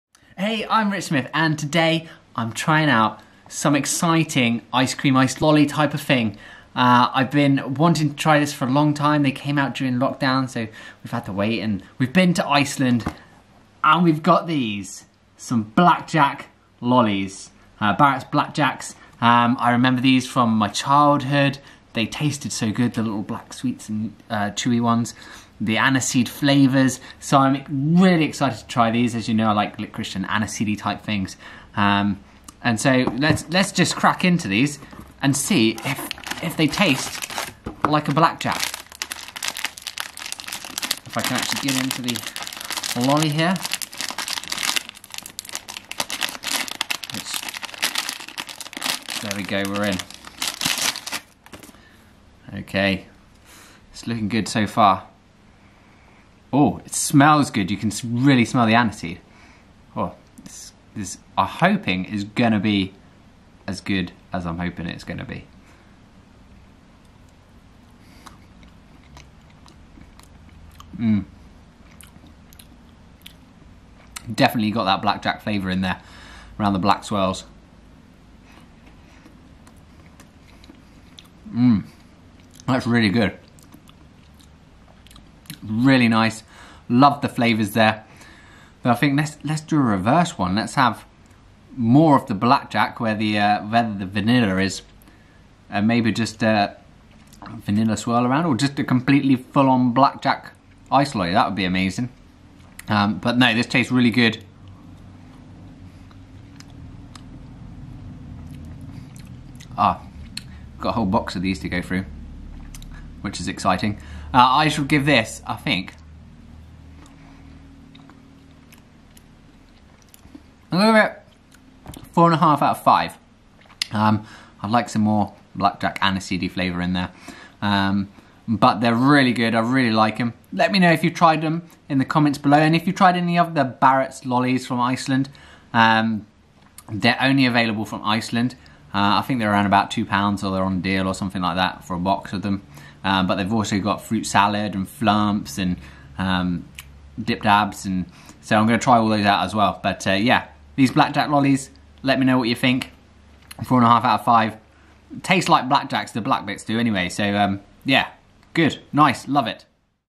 Taste Test - Iceland’s Barratt Black Jack Ice Lolly Review
Trying out Barratt Black Jack Ice Lollies exclusively from Iceland